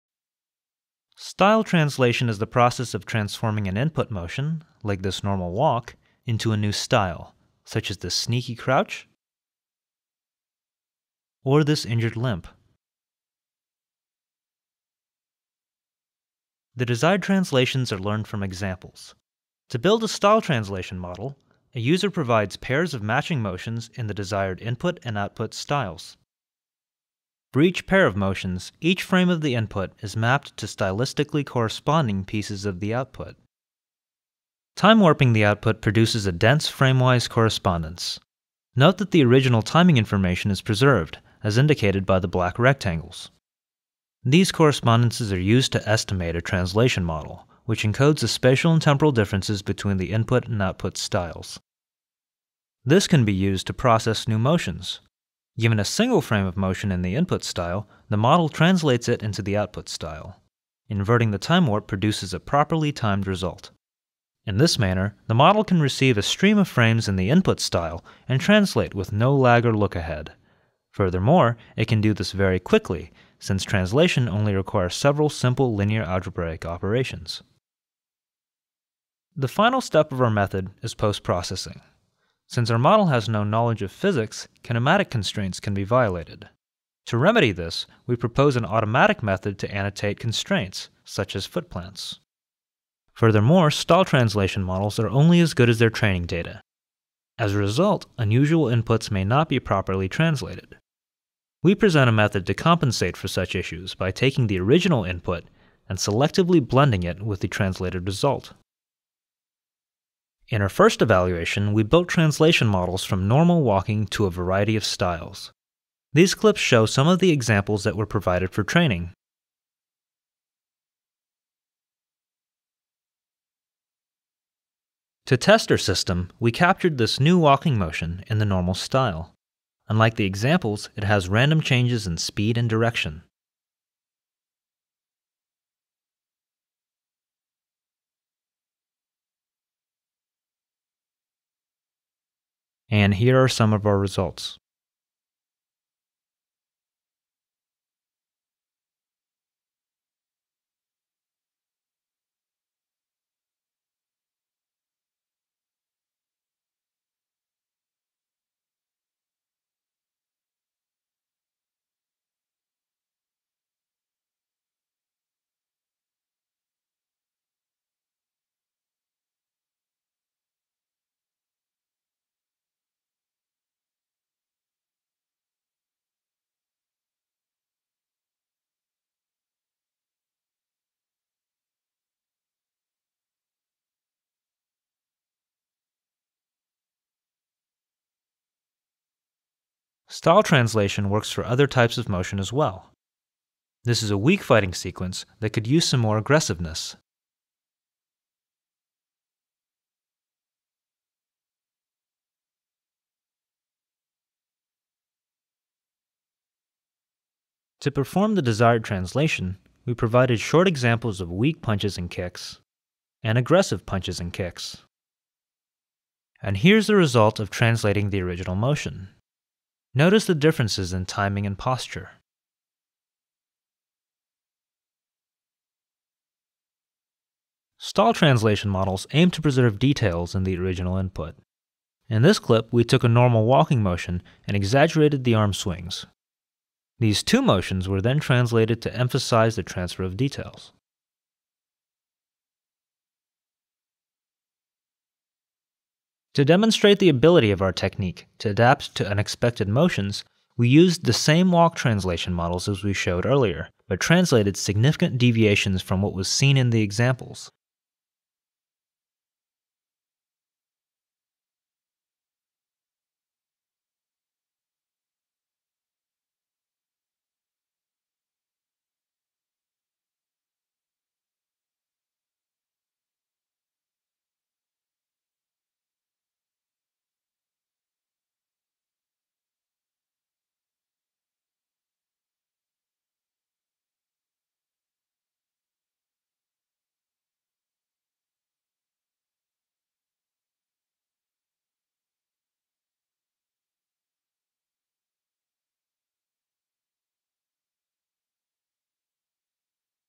Style Translation for Human Motion (Part 1)
Eugene Hsu, Kari Pulli, Jovan Popovic

Style translation is the process of transforming an input motion into a new style while preserving its original content. This problem is motivated by the needs of interactive applications, which require rapid processing of captured performances. Our solution learns to translate by analyzing differences between performances of the same content in input and output styles. It relies on a novel correspondence algorithm to align motions, and a linear time-invariant model to represent stylistic differences. Once the model is estimated with system identification, our system is capable of translating streaming input with simple linear operations at each frame.